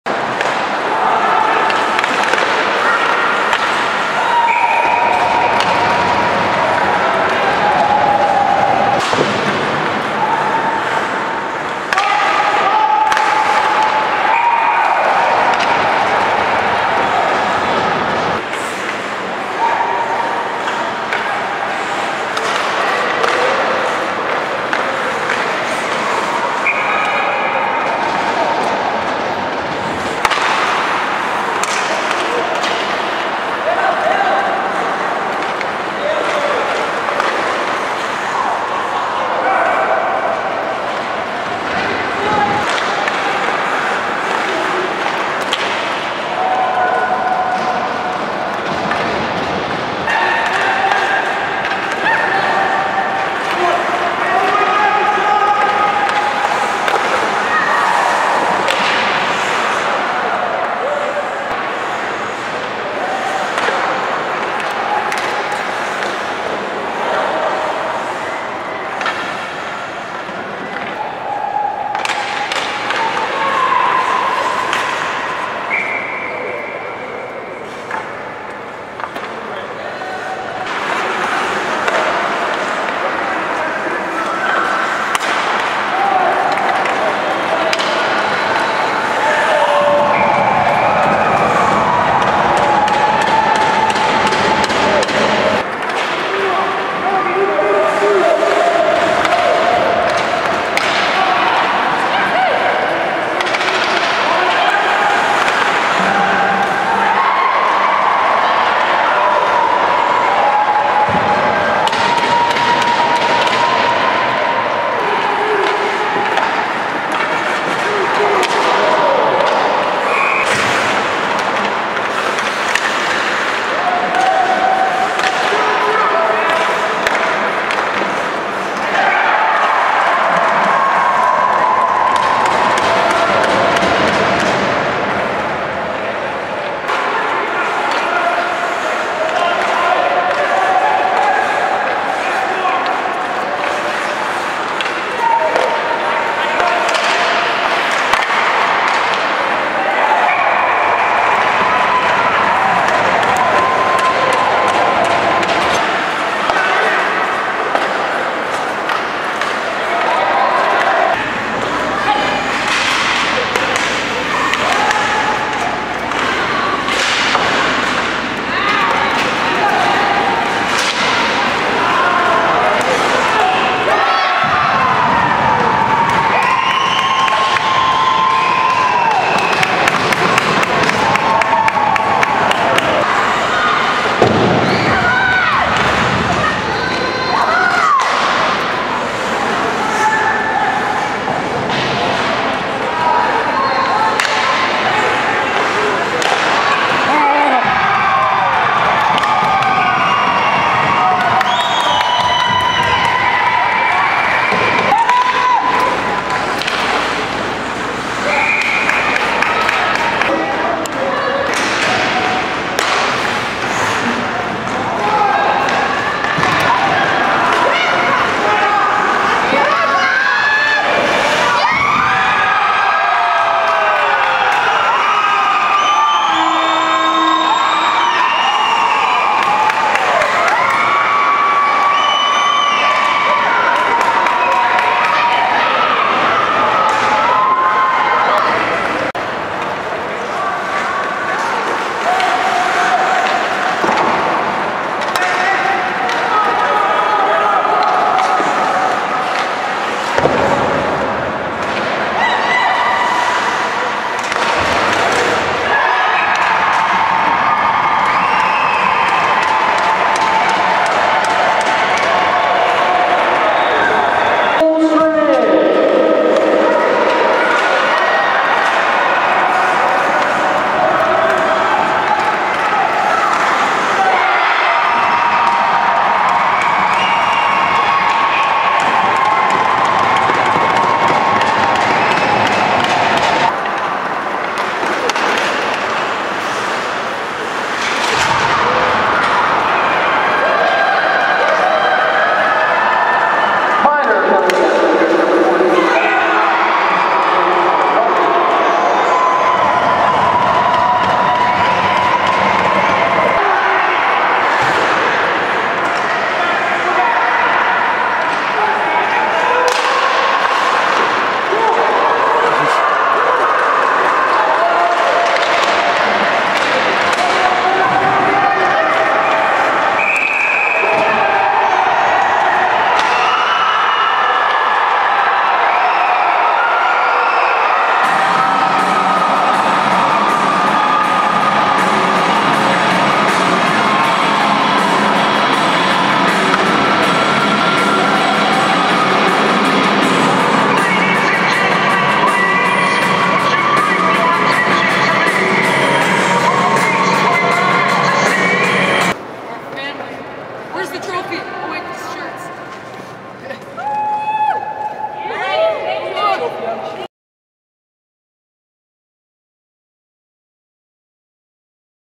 1997 MN Blades in the 2011 Walleye Chop
The 1997 MN Blades won the Walleye Chop hockey tournament in Minneapolis MN in 2011.